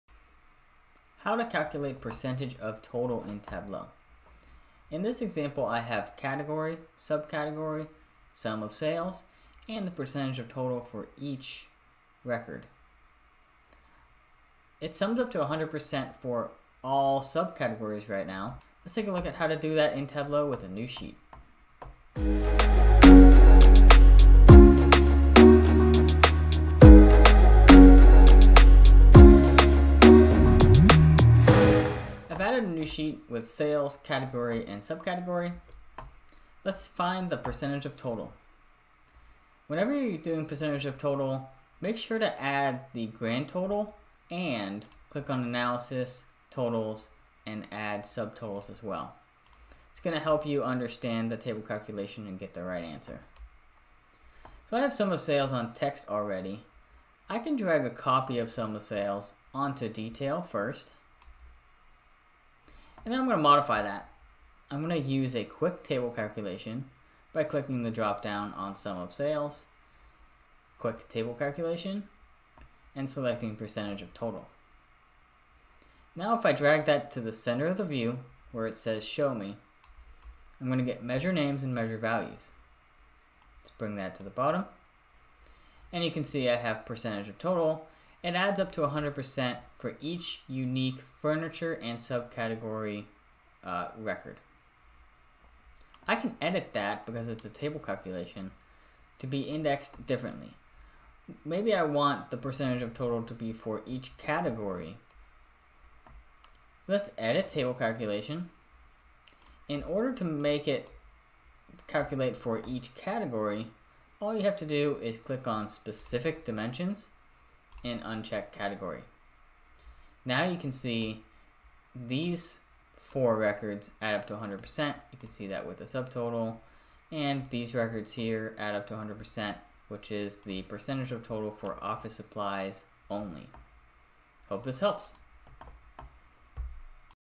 How to Workout Percentage of Total in Tableau
In this example I have category subcategory sum of sales and the percentage of total for each record. It sums up for all subcategories right now.

I've added a new sheet with sales category and subcategory. Let's find the percentage of total.
Whenever you're doing percentage of total make sure to add the grand total and click on analysis totals and add subtotals as well.

it's going to help you understand the table calculation and get the right answer. I have sum of sales on text already.  I can drag a copy of some of the sales onto detail first and then I'm going to modify that.

 I'm going to use a quick table calculation by clicking the drop down on sum of sales quick table calculation and selecting percentage of total. Now if I drag that to the center of the view where it says show me I'm going to get measure names and measure values. Let's bring that to the bottom and you can see I have percentage of total it adds up to 100 for each unique furniture and subcategory record.

 I can edit that because it's a table calculation to be indexed differently maybe I want the percentage of total to be for each category. a table calculation in order to make it calculate for each category all you have to do is click on specific dimensions and unchecked category.  Now you can see these four records add up to 100.  You can see that with a subtotal and these records here add up to 100 which is the percentage of total for office supplies only.